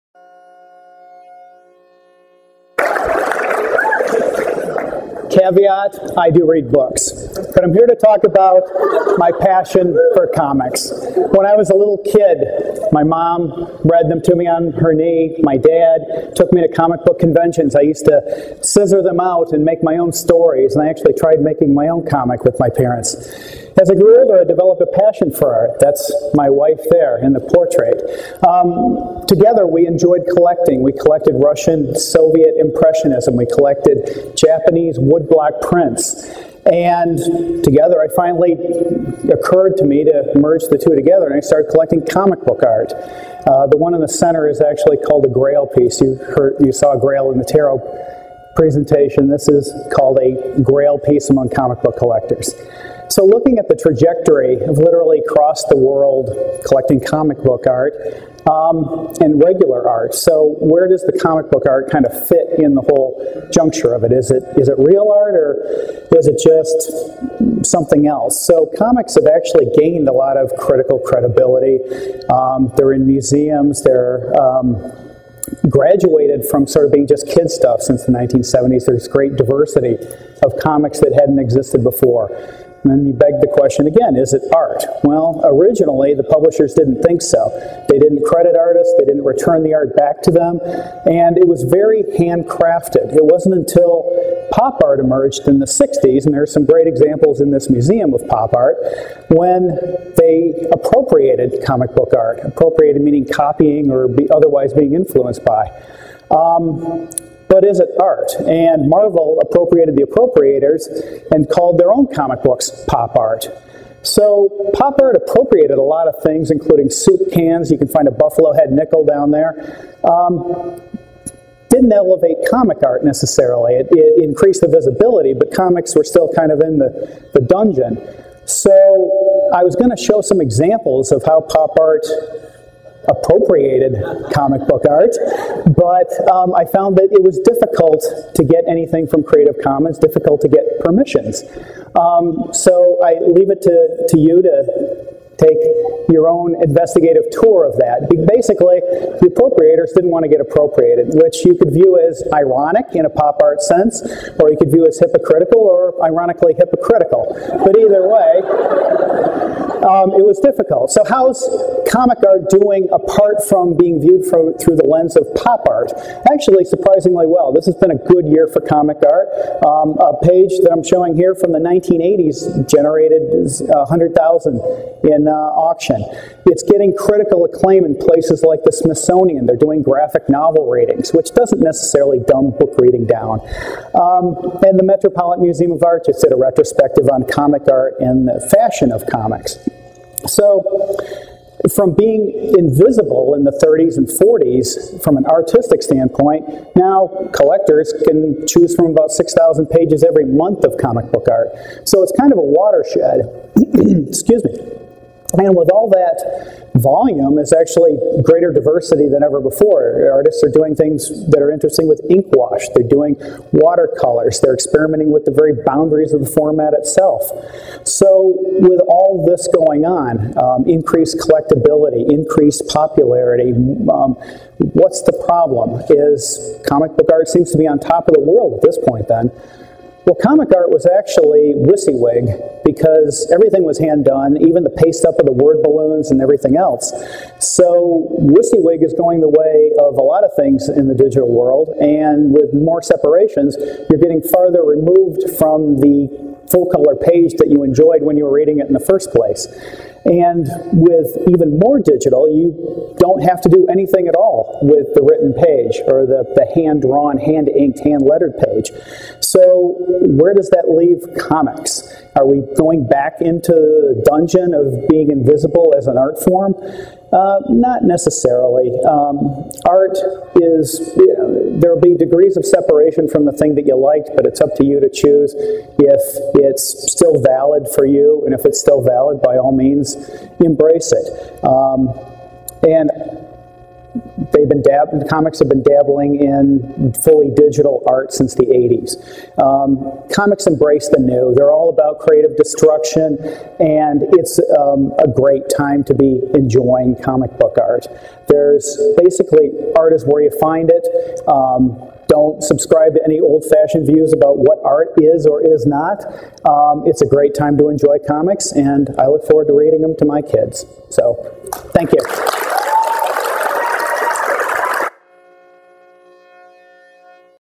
Ignite Phoenix #7 - The Art of Comics ... Pop Going, Going, Gone?
Presenter:  Kevin E Patterson

Biography:  Marketing manager for a Fortune 100 company who has been collecting art, including comic book art, for over twenty years.

Description:  Comic book art is the original Pop Art. Once pulped by publishers after printing, now sought after and legitimized by museums, collectors, and critics. Yet artists rely increasingly on computer technology and what appears on the page is literally vanishing. A look at the rise and creative destruction of comic art.